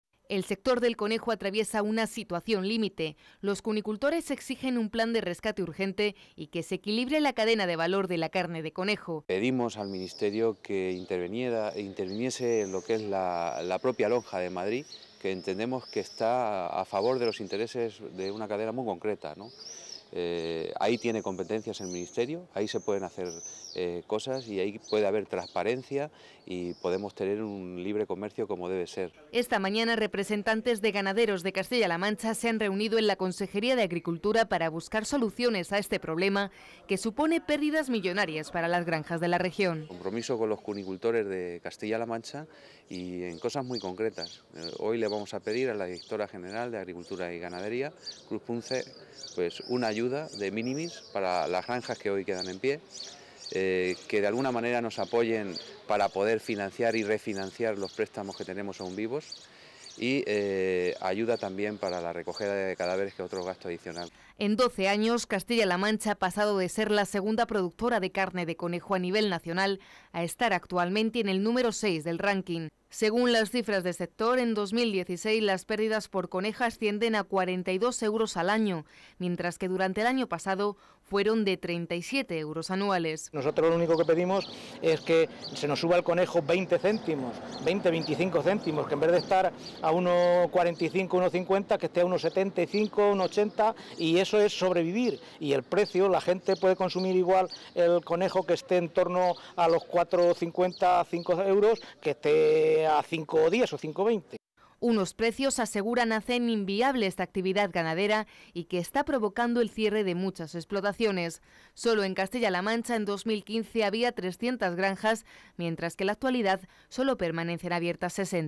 LOS CUNICULTORES PIDEN UN PLAN DE RESCATE PARA SALVAR EL SECTOR
El sector de la carne de conejo de Castilla-La mancha lleva más de 30 meses en crisis. Los cunicultores exigen al Gobierno Central un Plan de Rescate que salve a este sector de la ruina. Esta mañana, representantes ganaderos se han reunido en la Consejería de Agricultura para pedir soluciones para que al menos se cubran los costes de producción.